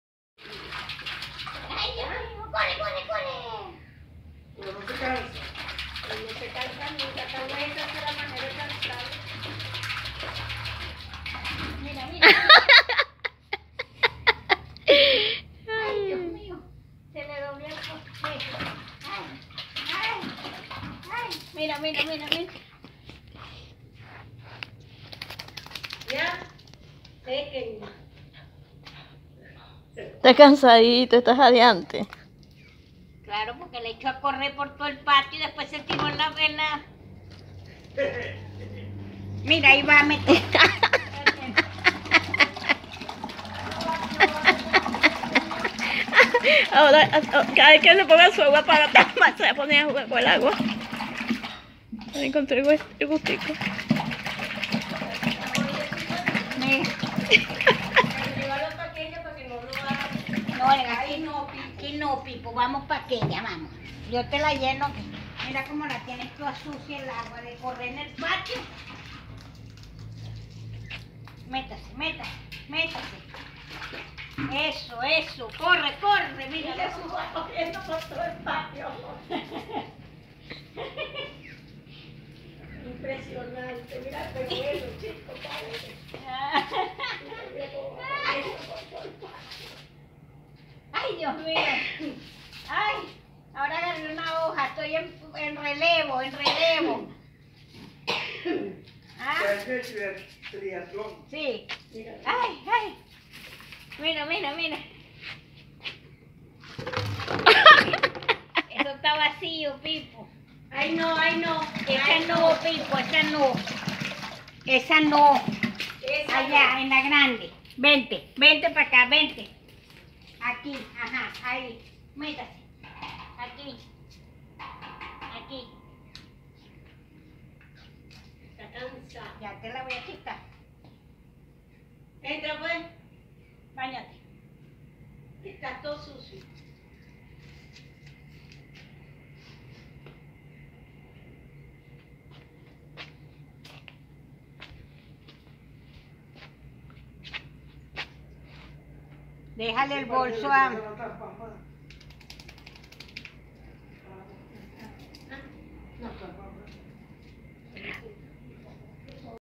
Pipo y su piscinada
Mientras hau quienes se quejan de que sus cachorros no se dejan bañar, miren a Pipo, un cachorrito que adora chapotear en el agua.